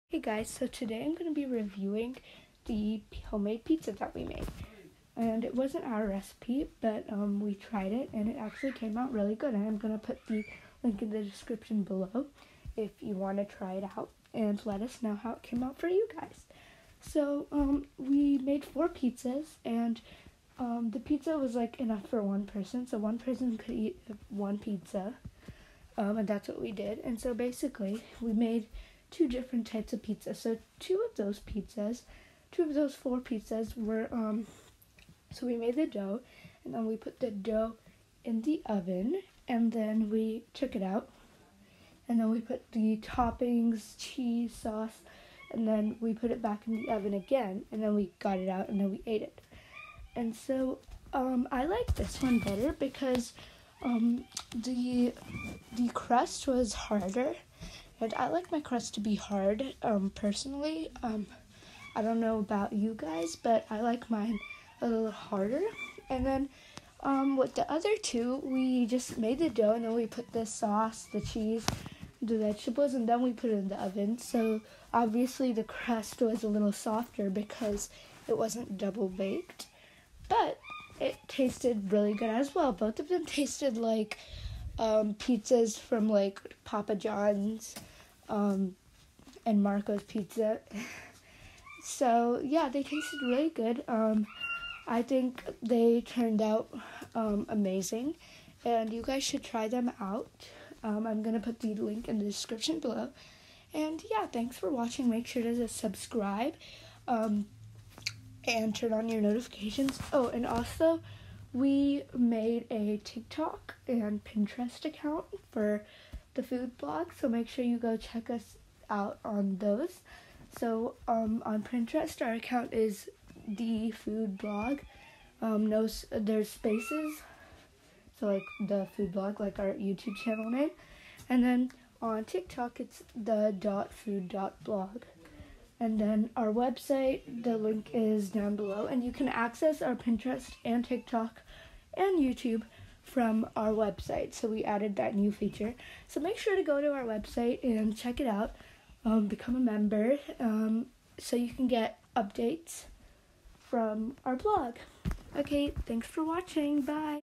HOMEMADE PIZZA REVIEW
hey guys! today i reviewed a homemade pizza recipe! 🍕